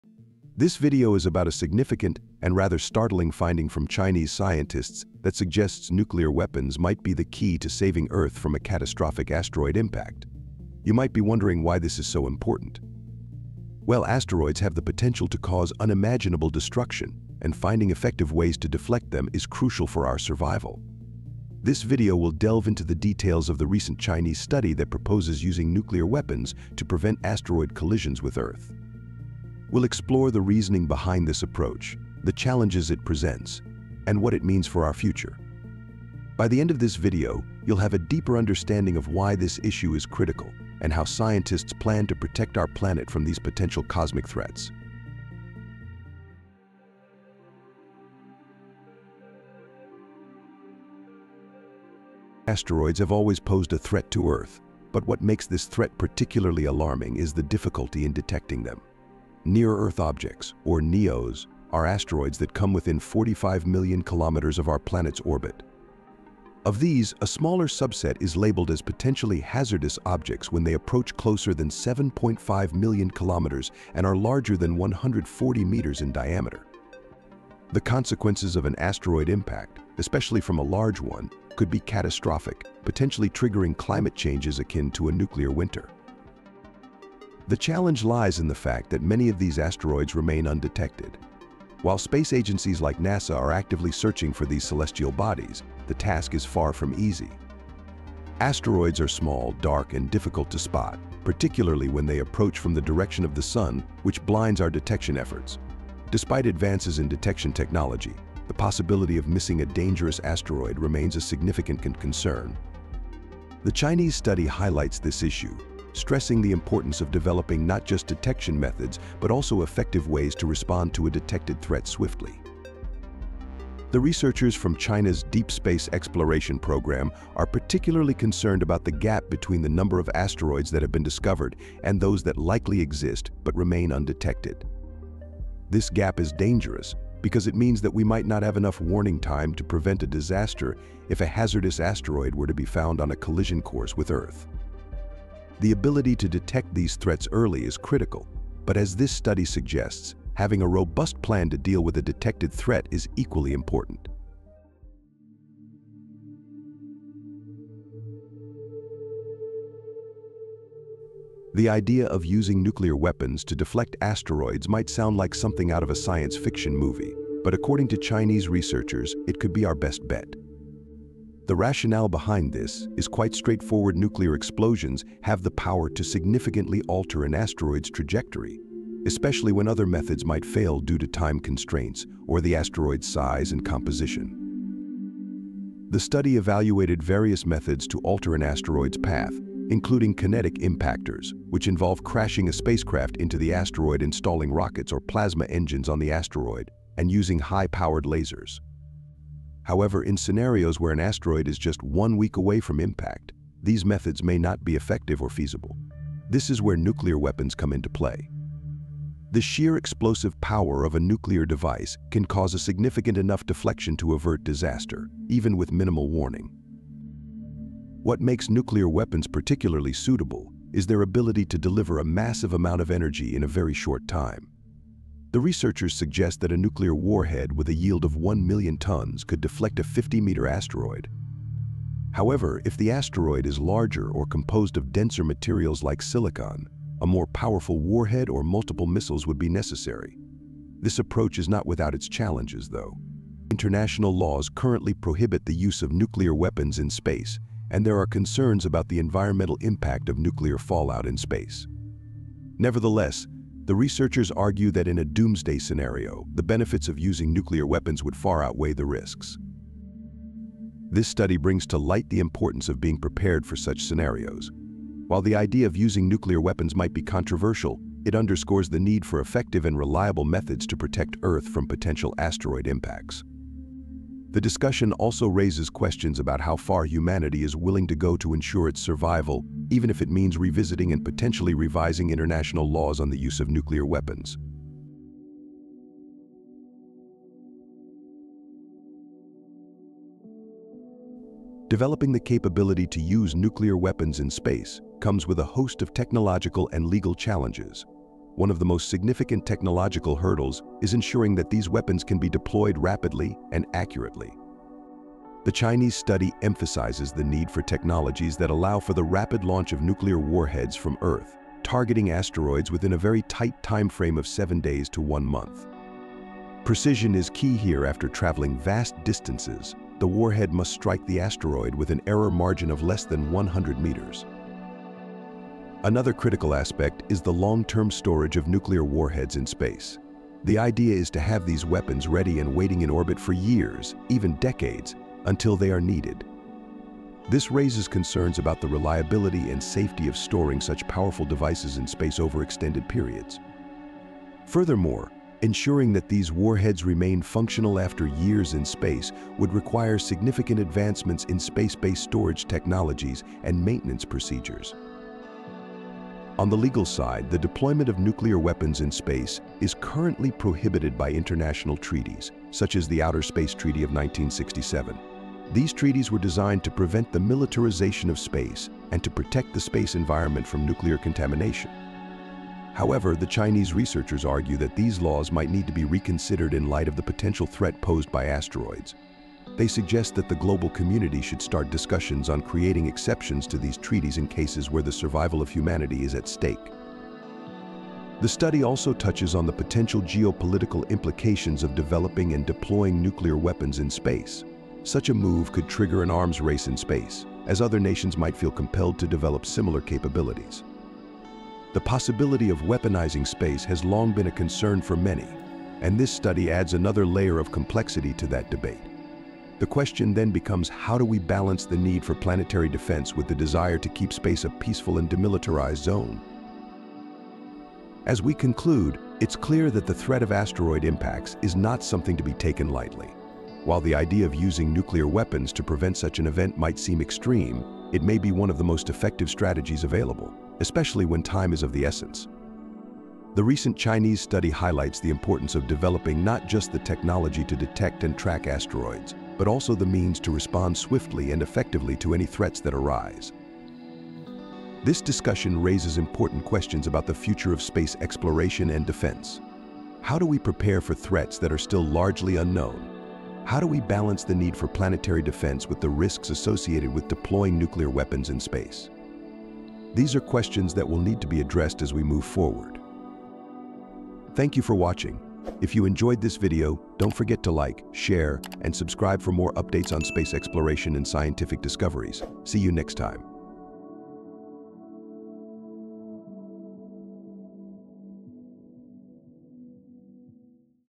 Nuclear Weapons: China's key to saving Earth from a catastrophic asteroid impact!
“Nuking: China’s Bold Plan Against Killer Asteroids!“
In this video, we explore a groundbreaking study by Chinese scientists that proposes using nuclear weapons as our last line of defense against asteroid threats. With millions of near-Earth objects out there, the risk of a catastrophic impact is real—and when time is running out, nuclear weapons might be the only way to save Earth. We’ll dive into the science behind this bold strategy, the challenges of deploying nukes in space, and the implications for our planet’s future. If you’re fascinated by space, science, or the potential threats we face from the cosmos, this is an episode you won’t want to miss. Learn how we might defend Earth from a doomsday asteroid scenario!